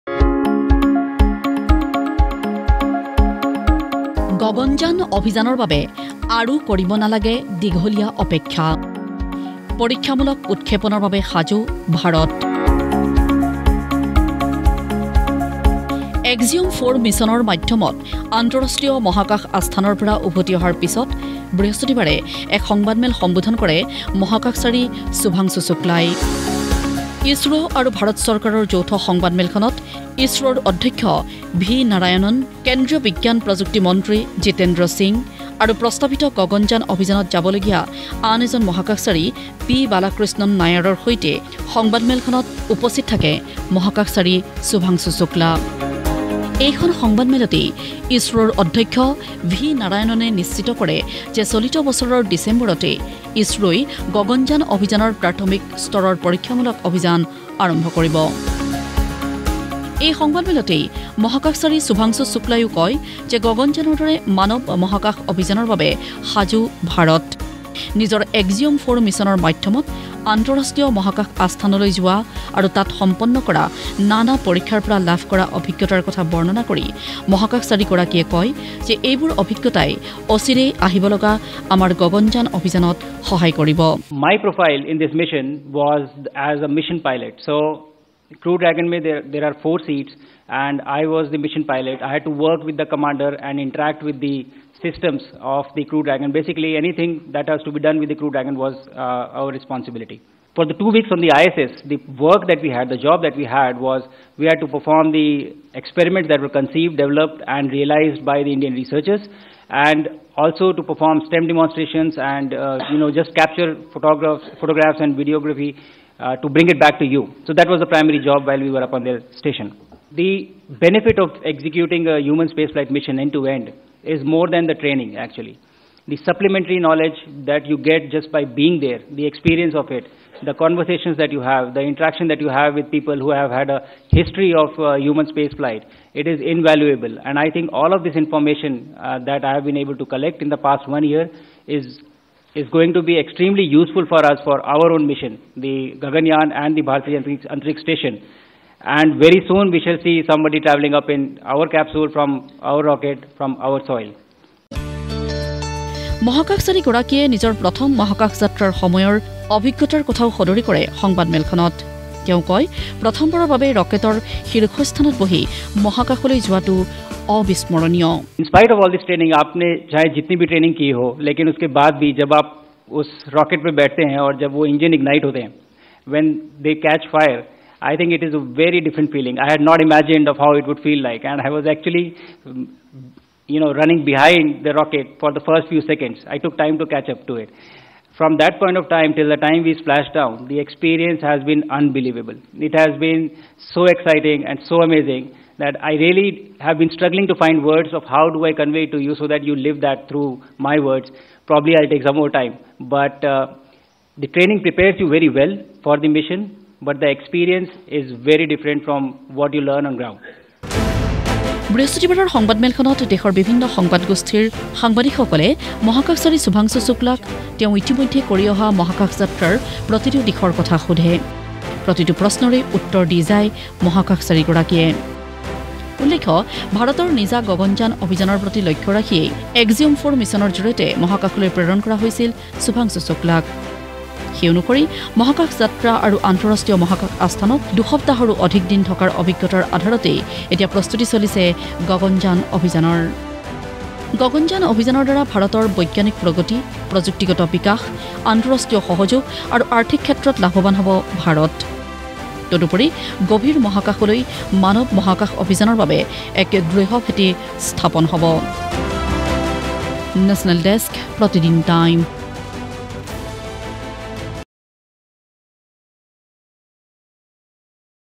চলিত বৰ্ষৰে অন্তিম ফালে ইছৰোই সম্পন্ন কৰিব বহু প্রত্যাশিত গগনযানৰ পৰীক্ষামূলক উৎক্ষেপণ।
চলিত বৰ্ষৰে অন্তিম ফালে ইছৰোই সম্পন্ন কৰিব বহু প্রত্যাশিত গগনযানৰ পৰীক্ষামূলক উৎক্ষেপণ। আজি ইছৰো আৰু কেন্দ্রীয় চৰকাৰৰ এক যৌথ সংবাদমেলত সদৰী কৰা হয় এই কথা। ইছৰোৰ অধ্যক্ষ ভি. নাৰায়ণন আৰু মহাকাশচাৰী শুভাংশু শুক্লা উপস্থিত থকা সংবাদমেলখনত কোৱা হয় যে মানৱ মহাকাশ অভিযানৰ বাবে সাজু ভাৰত।